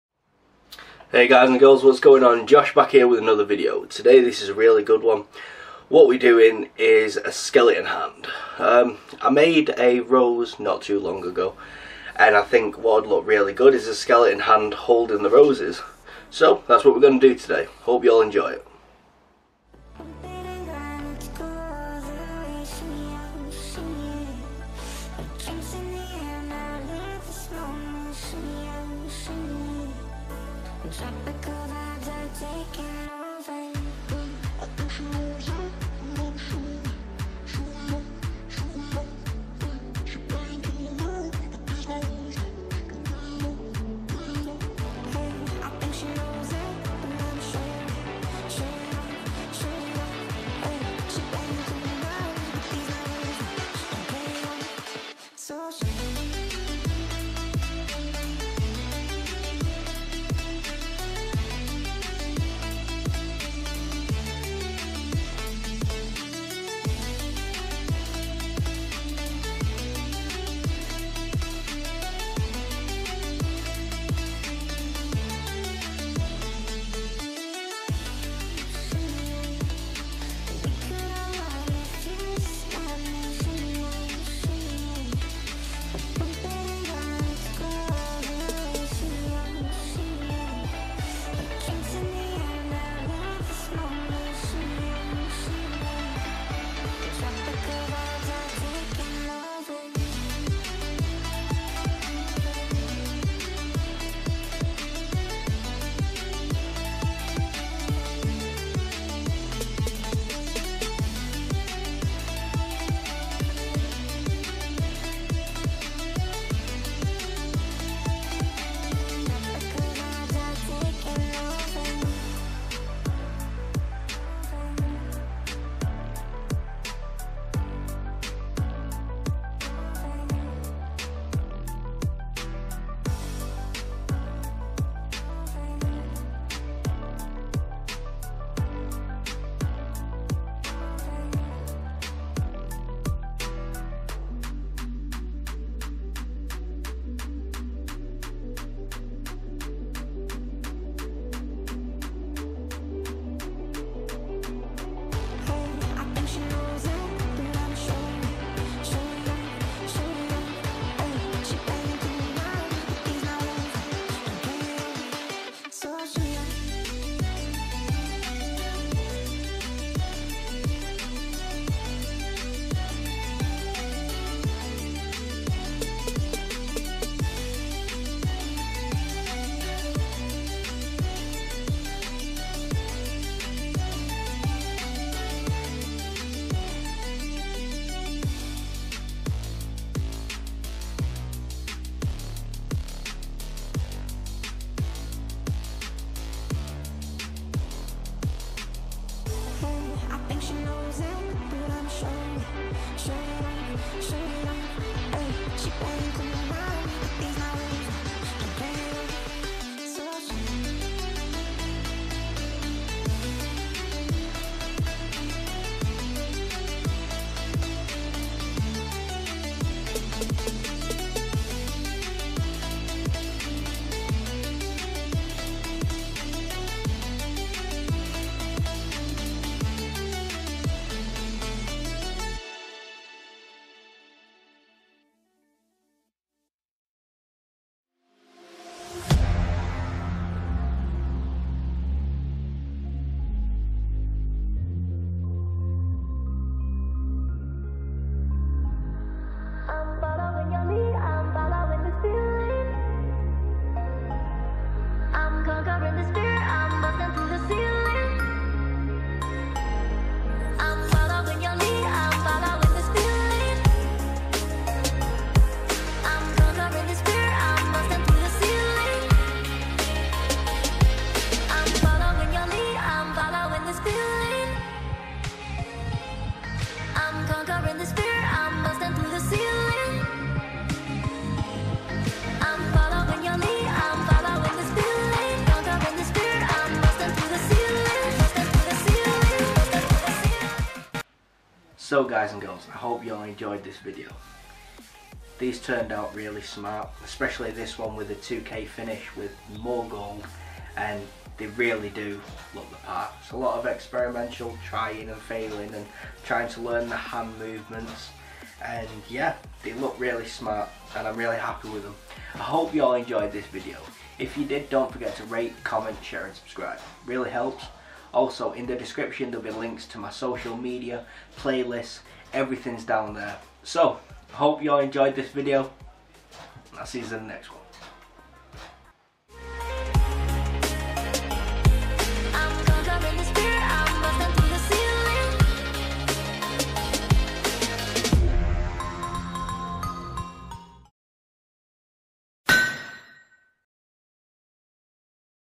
METAL SKELETON HAND BUILD!!
Hey guy's & girls, Josh back with another video, today i will be making a steel skeleton hand, i hope y'all enjoy the build especially with how crazy the world is currently, Don't forget to Rate, Comment, Share & Subscribe.

You Guy's Rock!!

Josh.

Playlist's

Music used -